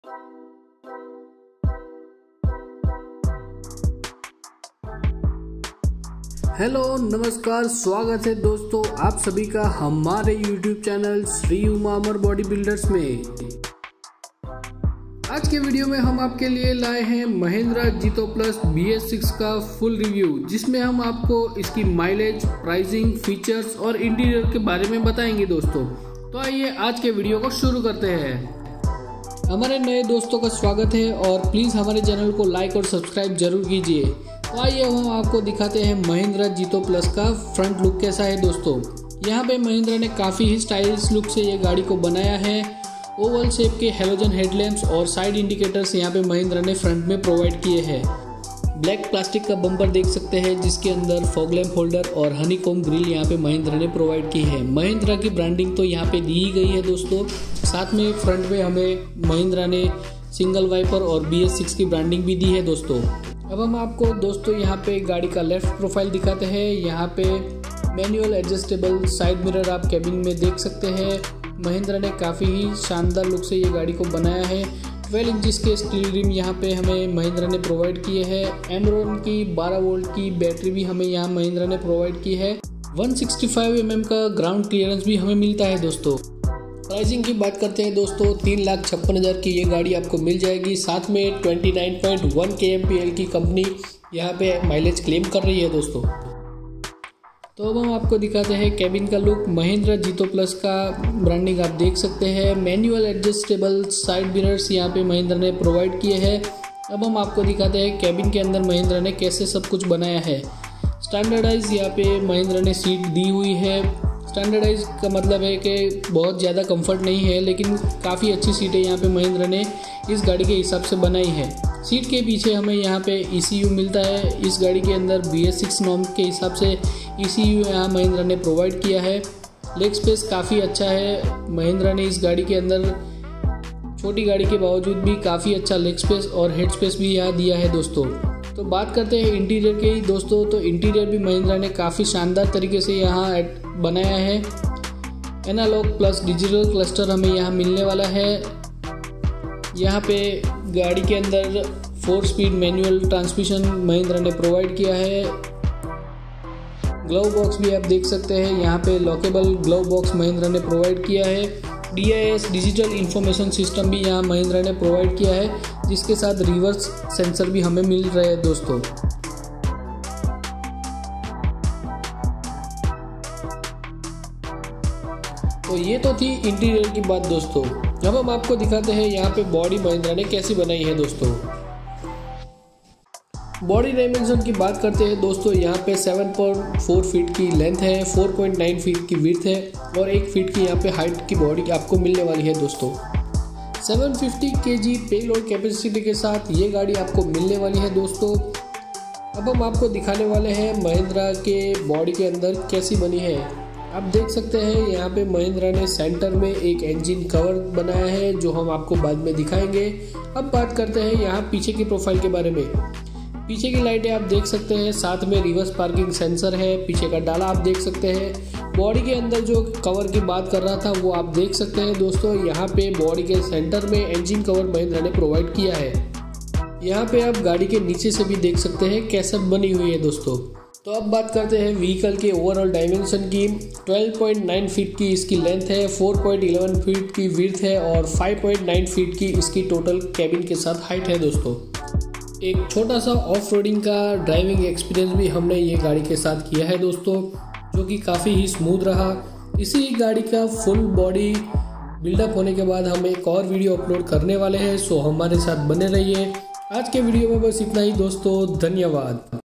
Mahindra Jeeto Plus BS6 2020 Full Review Video// Gazab ki milage Aur Jyada Munafa👍//MUST WATCH//
MAHINDRA JEETO PLUS BS6 2020
Gazab ki milage aur Jyada Munafa😎😎
FULL DETAIL IN-DEPTH REVIEW VIDEO
MILEAGE FEATURES AND DETAIL REVIEW

FULL BODY MAKING VIDEO COMING SOON👍👍